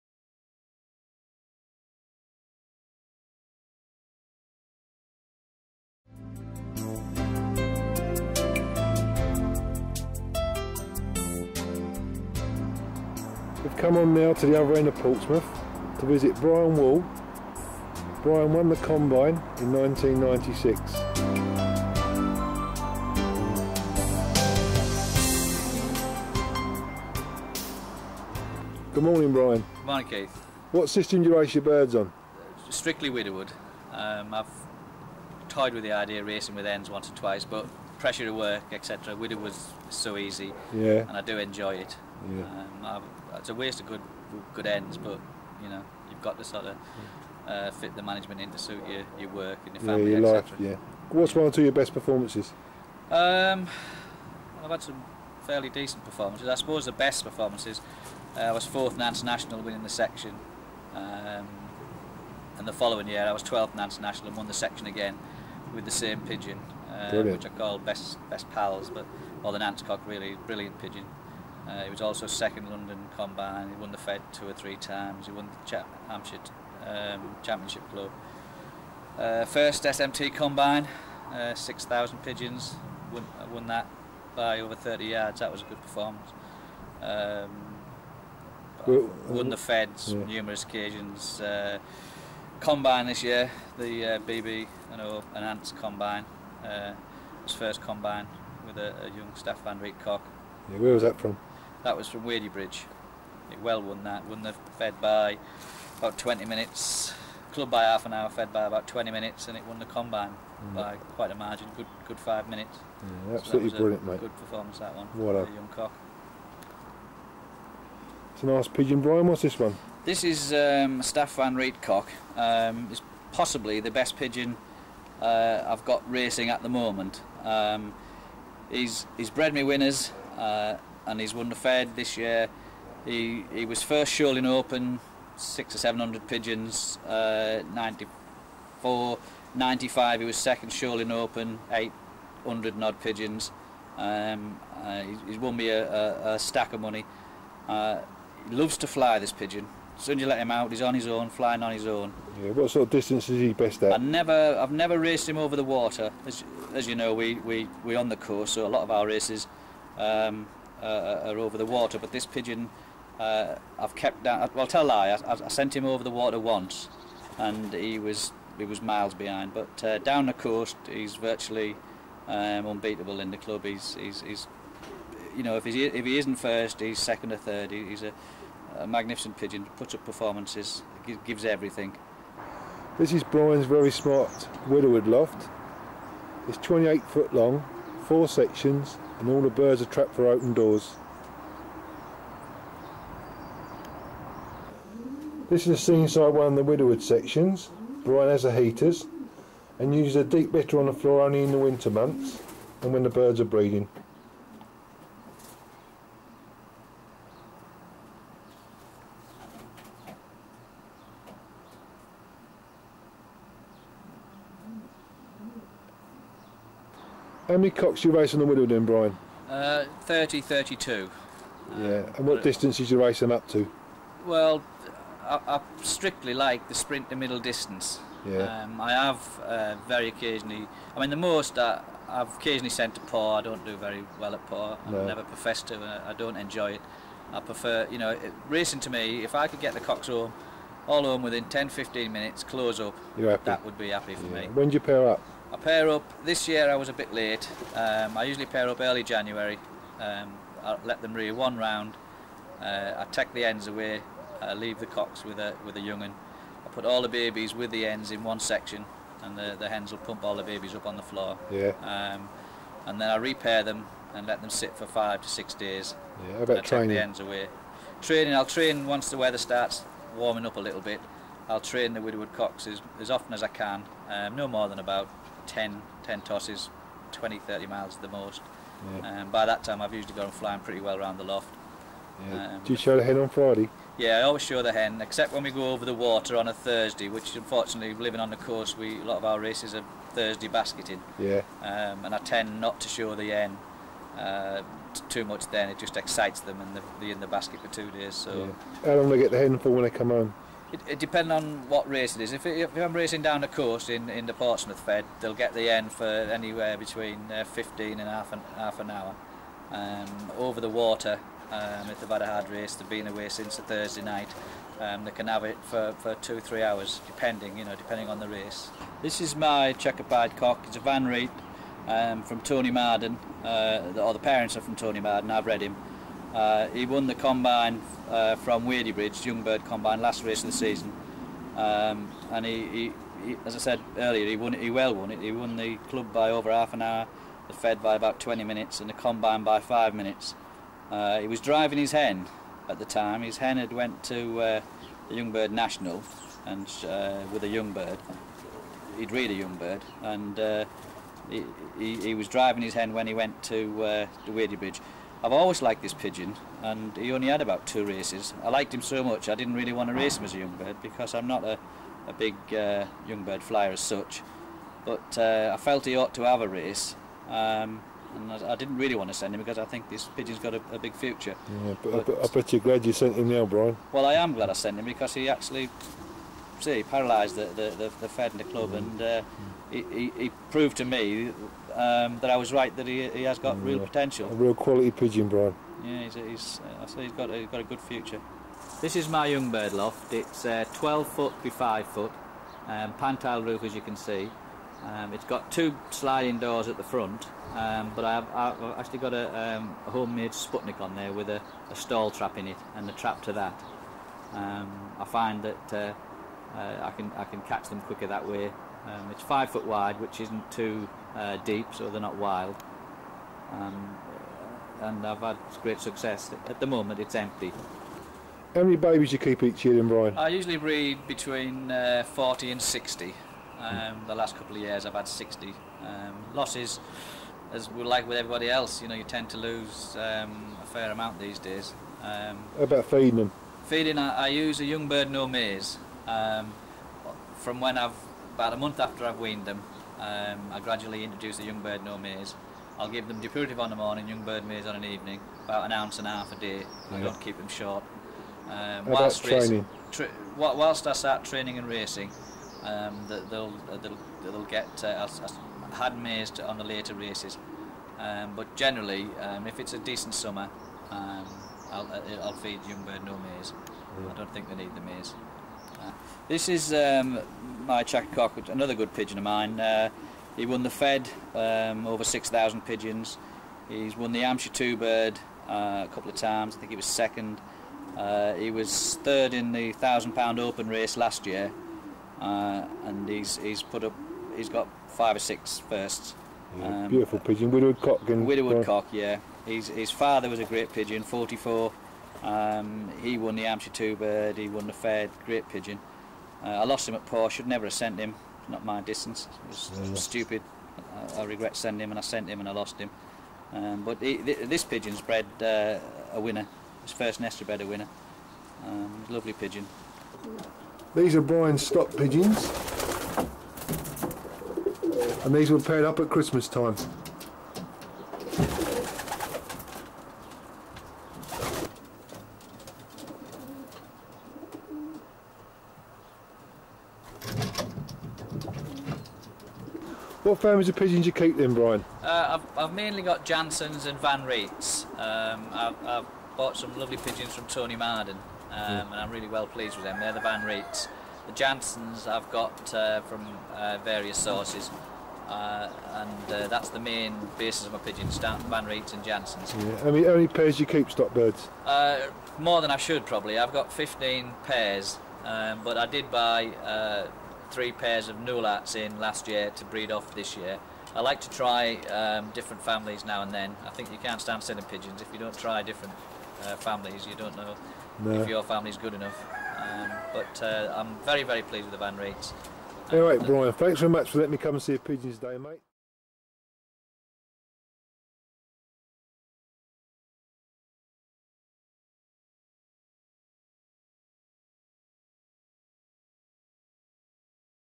Video 209: Brian Wall of Portsmouth: Premier Pigeon Racer
Brian Wall, one of the top pigeon racers in the south of England, winning it all including 1st open Combine. A great pigeon man and a 'GEM' of a loft visit.